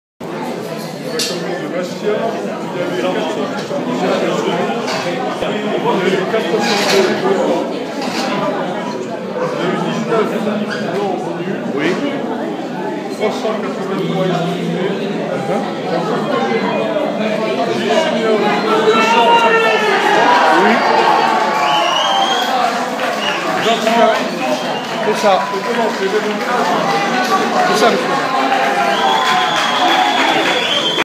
annonce resultats bureau centralisateur
Dimanche 30 Mars vers 19h
Annonce du résultat du bureau 1 Bastia
Après 5 bonnes minutes d'attente pour pouvoir annoncer le résultat dans de bonnes conditions, Emile se lance...